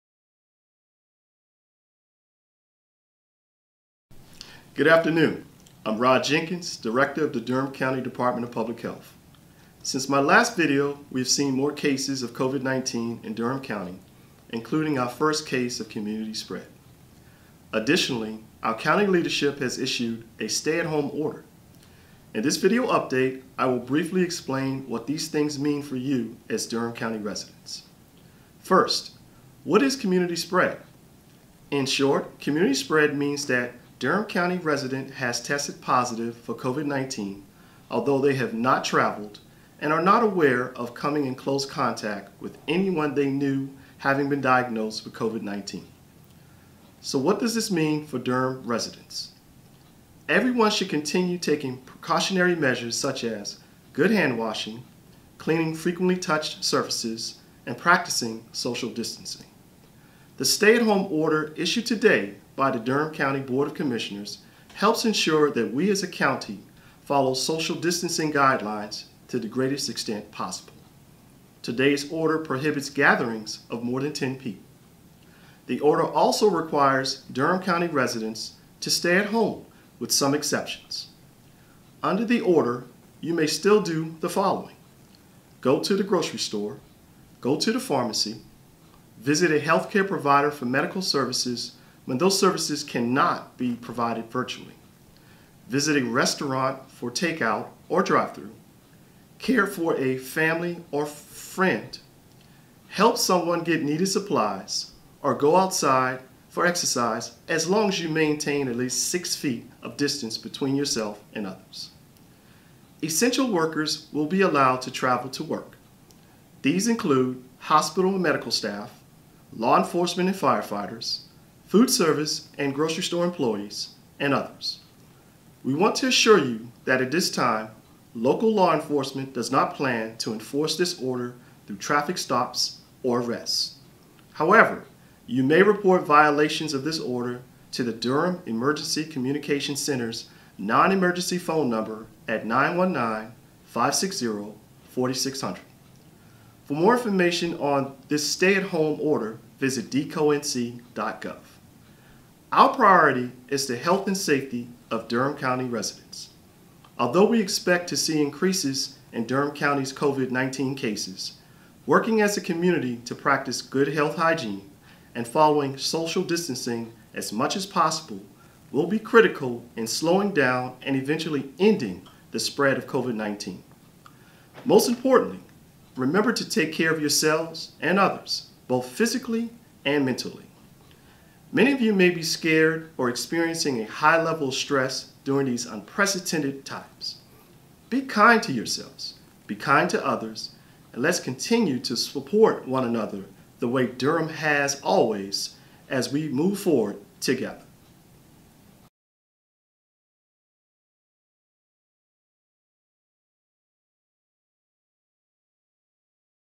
3/28 COVID-19 Update - Durham County Department of Public Health
Community spread, the Durham County Stay-at-Home Order, and more COVID19 information in our latest video update from Health Director Rod Jenkins.